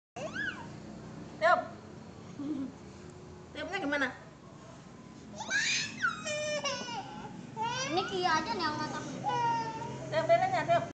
inflatable candles 03.08.2014
Happy birthday, happy birthday,, happy birthday my twin.
My twin girls first birthday, mommy love you all :) as always